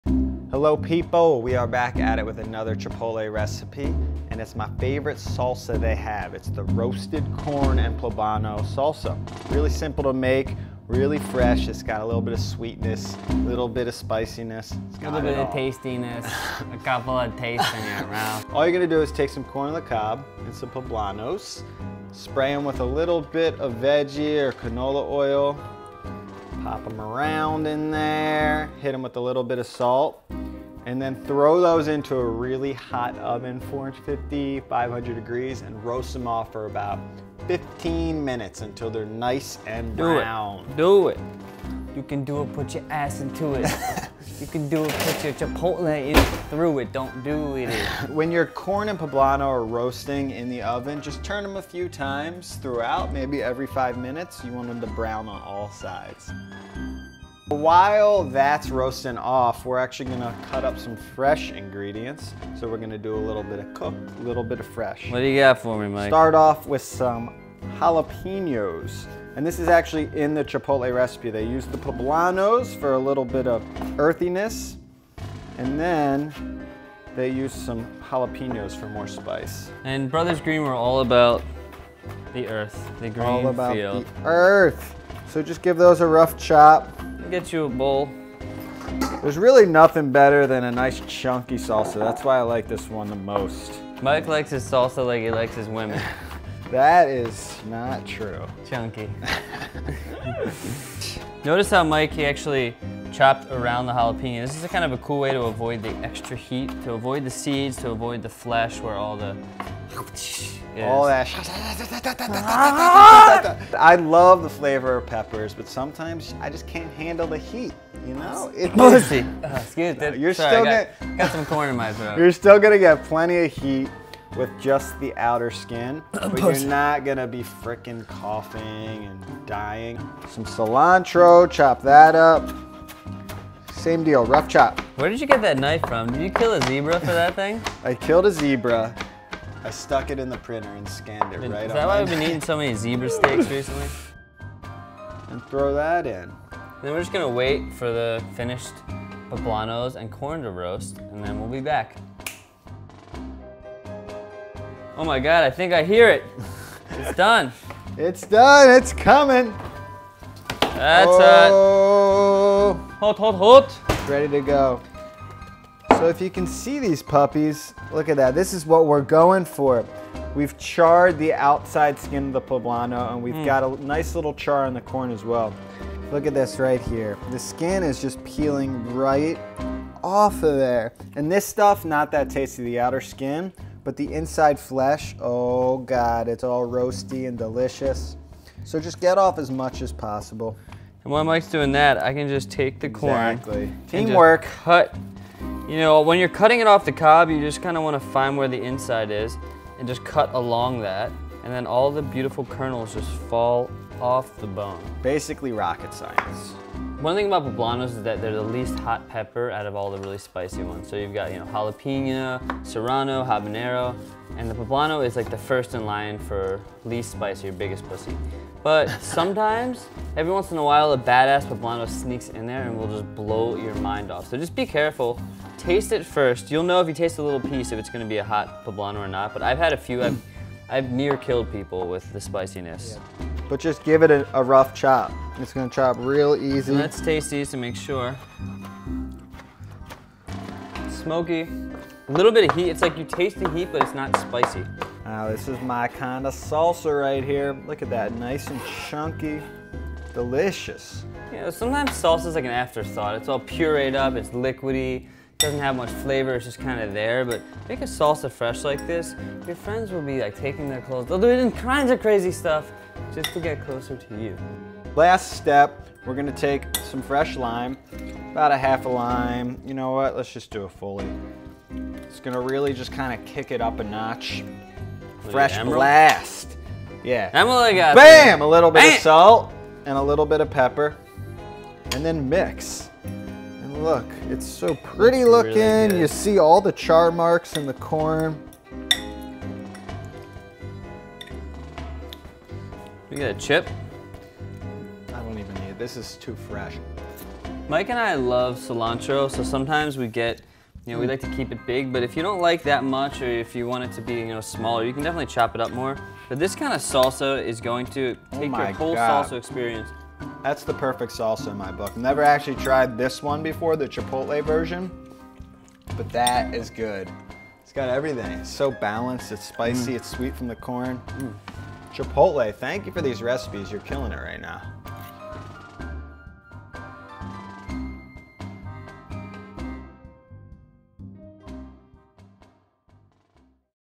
Chipotle's Chili-Corn Salsa - Roasted Poblano - Recipe
Getting your kitchen gear from Pro Home Cooks supports more content like this. Browse below!